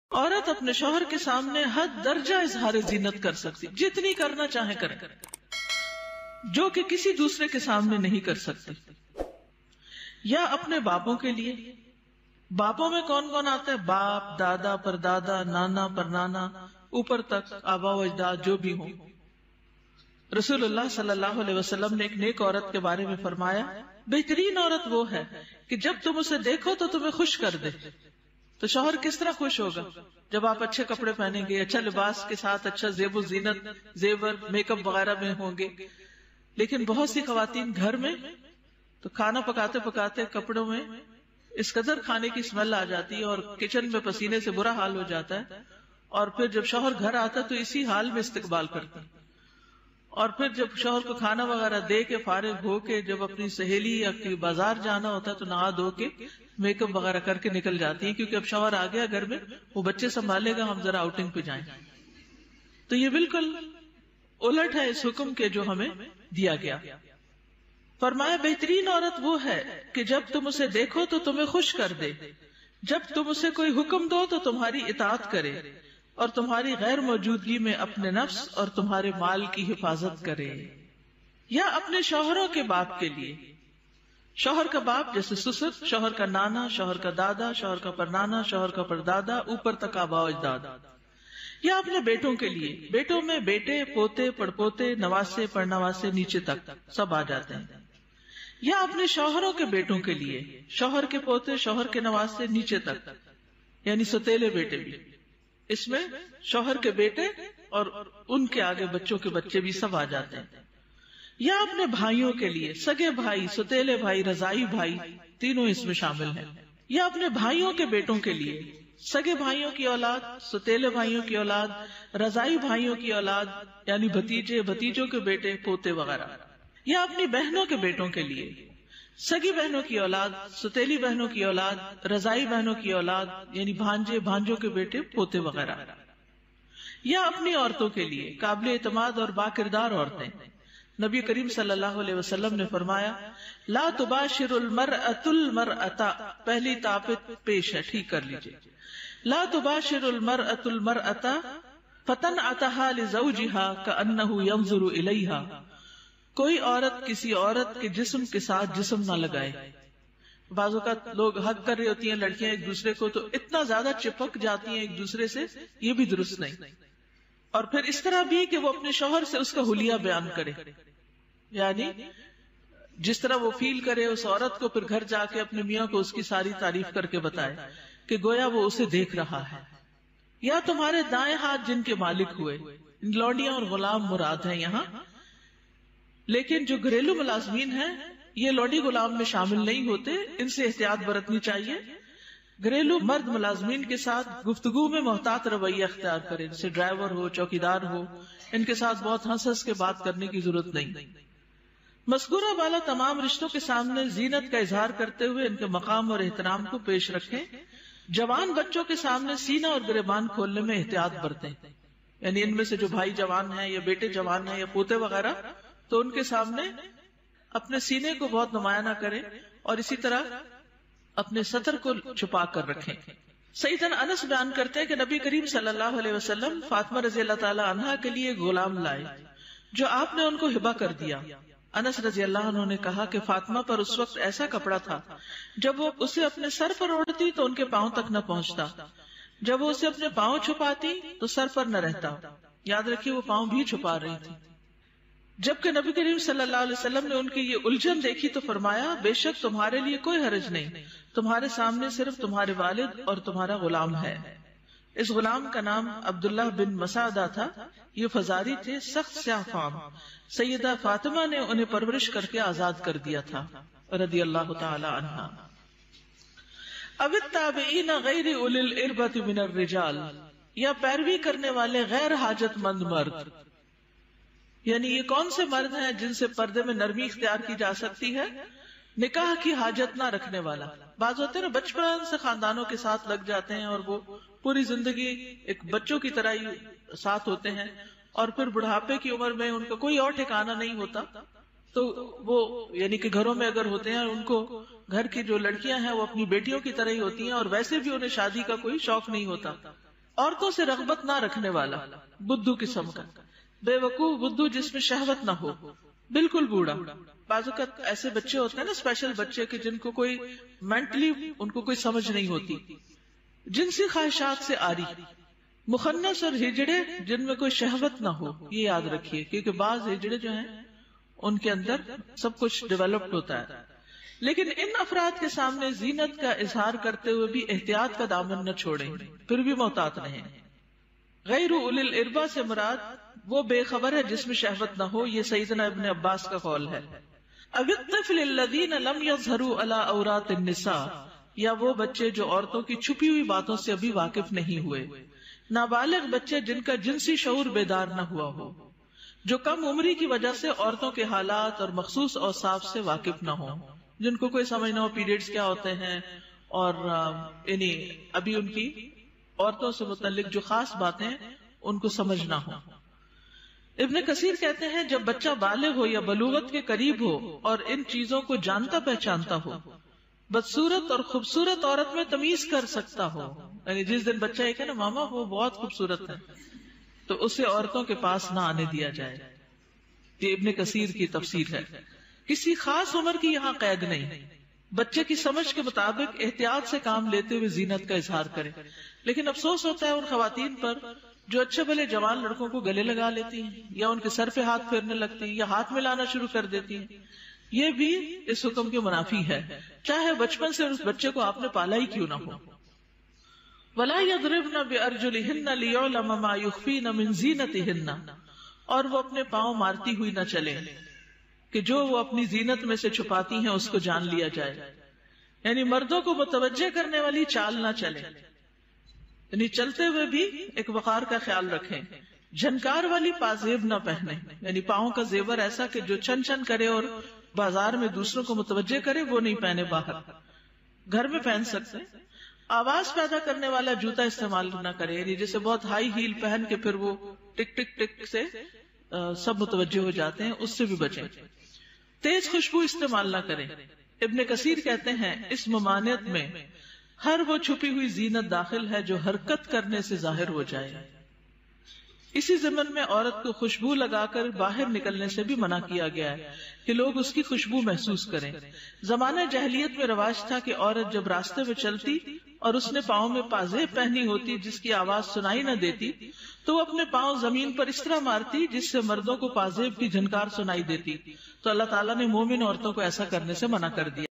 Aurton Ka Gale Milna | By Dr Farhat Hashmi
Aurton Ka Gale Milna" is a thought-provoking Urdu lecture by Dr. Farhat Hashmi, exploring the significance and cultural aspects of women embracing each other in a gesture of friendship and camaraderie. Dr. Farhat Hashmi delves into the Islamic perspective on social interactions among women, providing insights into the etiquettes and meanings associated with expressing affection through gestures like hugging. This lecture aims to foster a deeper understanding of the nuances of social conduct for women in light of Islamic teachings.